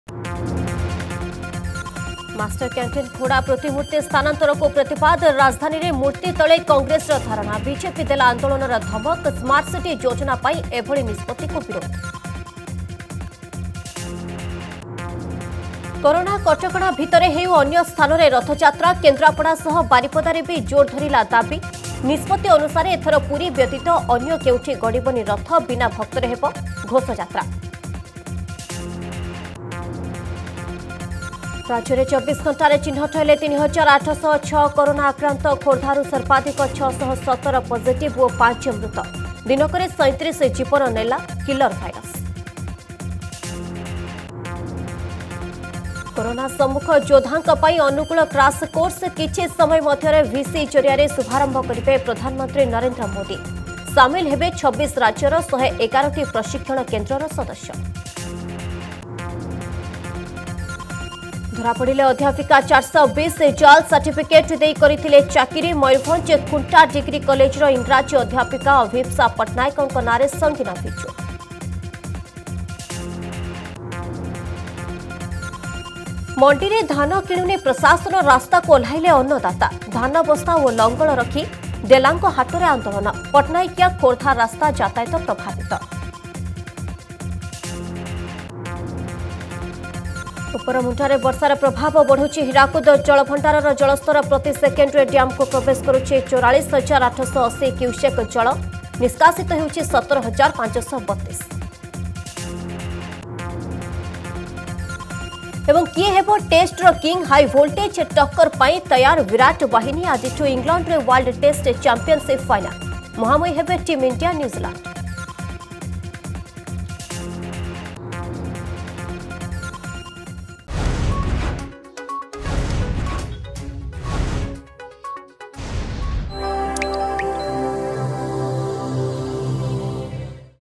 Headlines 11AM
+ ମାଷ୍ଟରକ୍ୟାଣ୍ଟିନ ଘୋଡ଼ା ପ୍ରତିମୂର୍ତ୍ତି ସ୍ଥାନାନ୍ତରକୁ ପ୍ରତିବାଦ
+ କରୋନା କଟକଣା ଭିତରେ ହେଉ ଅନ୍ୟ ସ୍ଥାନରେ ରଥଯାତ୍ରା
+ ରାଜ୍ୟରେ ୨୪ ଘଣ୍ଟାରେ ଚିହ୍ନଟ ହେଲେ ୩,୮୦୬ କରୋନା ଆକ୍ରାନ୍ତ
+ କରୋନା ସମ୍ମୁଖ ଯୋଦ୍ଧାଙ୍କ ପାଇଁ ‘ଅନୁକୂଳ କ୍ରାଶ କୋର୍ସ
+ ଧରାପଡ଼ିଲେ ଅଧ୍ୟାପିକା 420